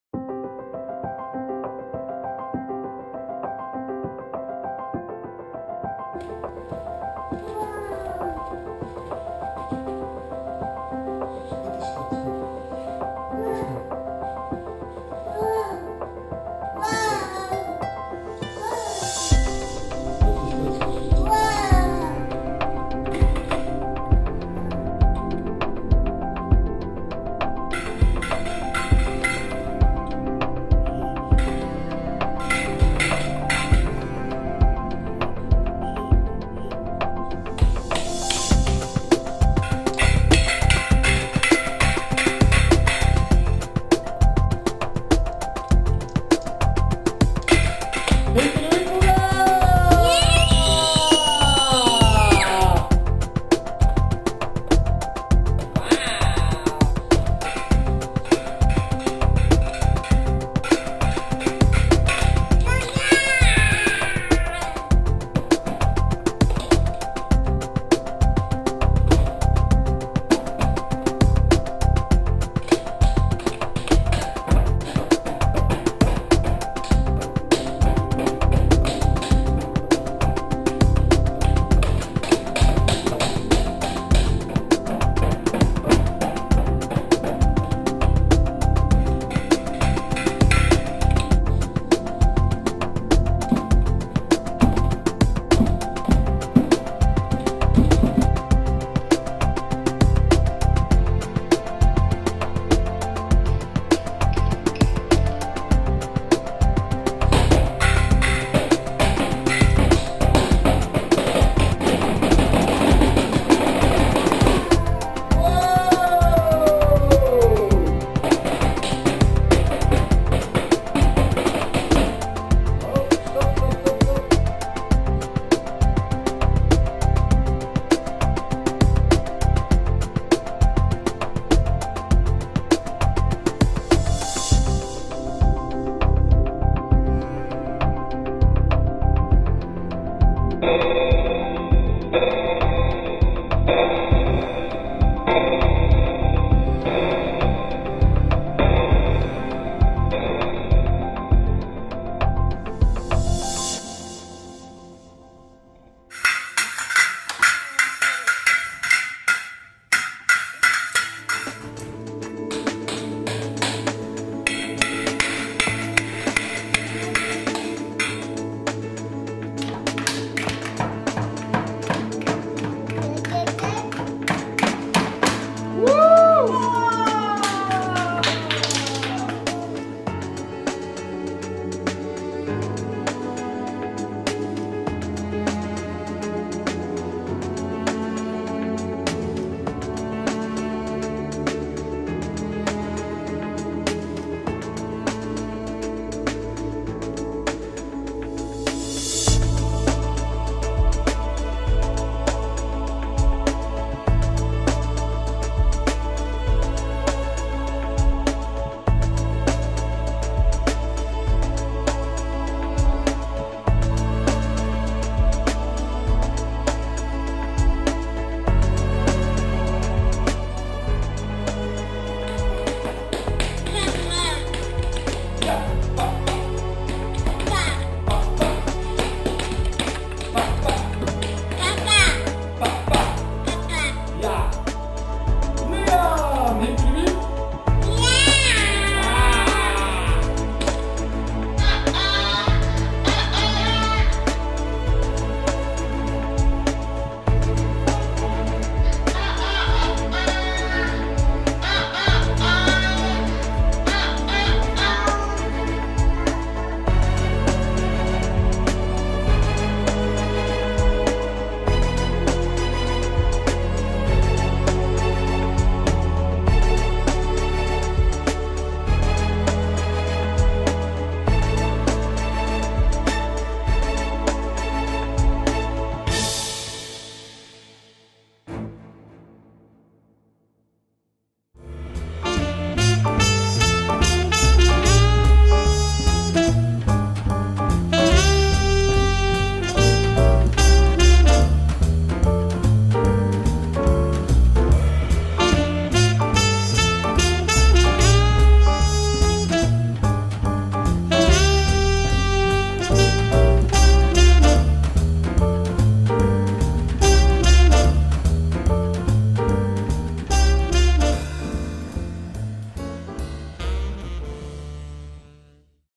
Our Little Drummer Boy HD
Our little boy got his first drum kit for Christmas.
Here you will see how he handles and likes it.